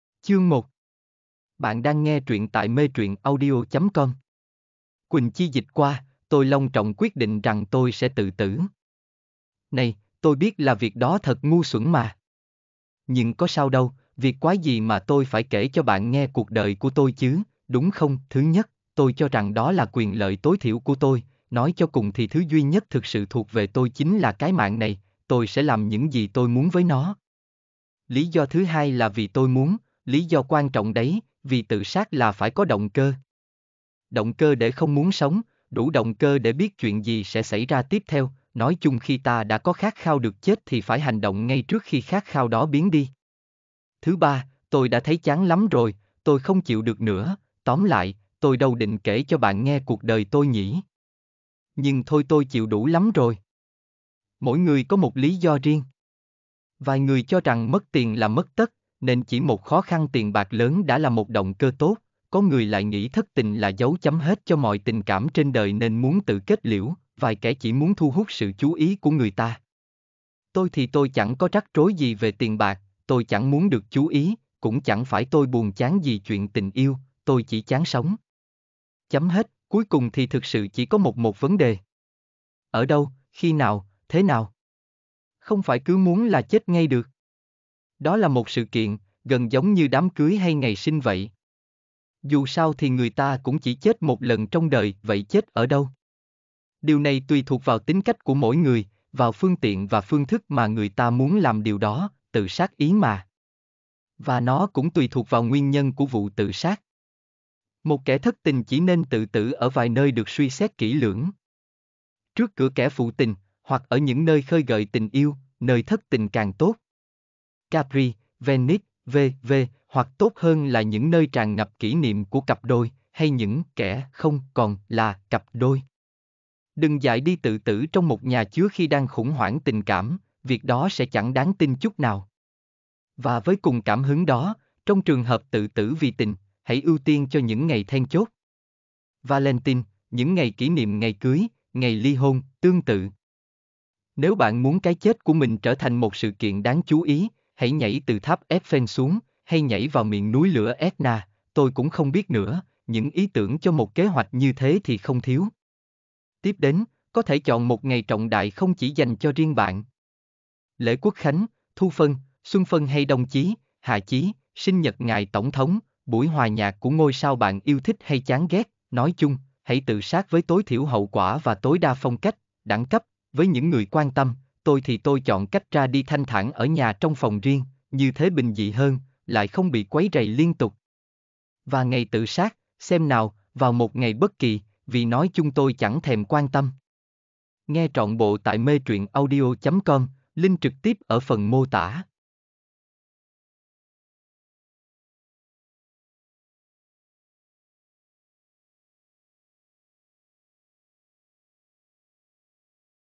Recette Du Suicide - Công Thức Tự Sát Audio Trọn Bộ - Chương 1 | Melina Dasgessas Đam Mỹ Đoản Văn
Truyện: RECETTE DU SUICIDE - CÔNG THỨC TỰ SÁT Tập 1
Tác giả: Melina Dasgessas
Thể loại: Đam Mỹ, Đoản Văn